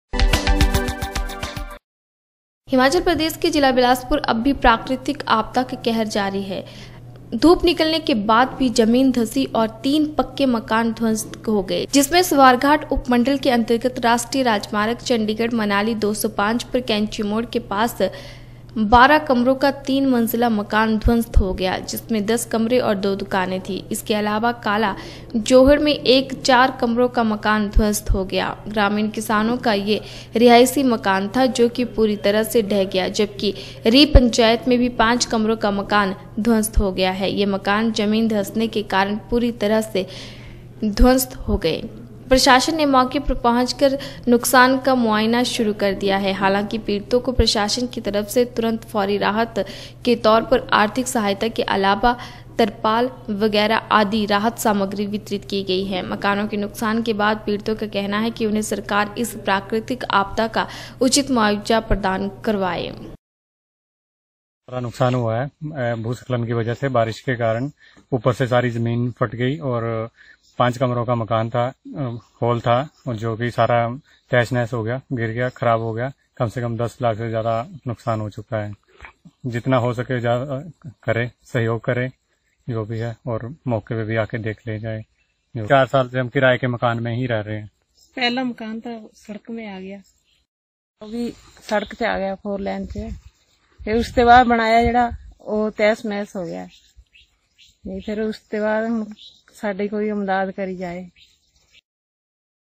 हिमाचल प्रदेश के जिला बिलासपुर अब भी प्राकृतिक आपदा का केहर जारी
हिमाचल प्रदेश के  जिला बिलासपुर  अब भी प्राकृतिक आपदा का केहर जारी हैं धूप निकलने के बाद धसी जमीने तीन रिहाशी पक्के मकान ध्वस्त जिनमें स्वारघाट उपमंडल के अंतर्गत राष्ट्रीय राज मार्ग चंडीगढ़ .मनाली 205 पर कैंची  मोड के पास आज सुबह 12 कमरों का तीन मंजिला मकान ध्वस्त हो गया  जिसमे 10 कमरे और दो दुकाने थी इसके अलावा  काला जोहड़ में एक चार कमरों का मकान ध्वस्त हो गया  ग्रामीण किसान का ये   रिहायशी मकान था जो की पूरी  तरह ढह गया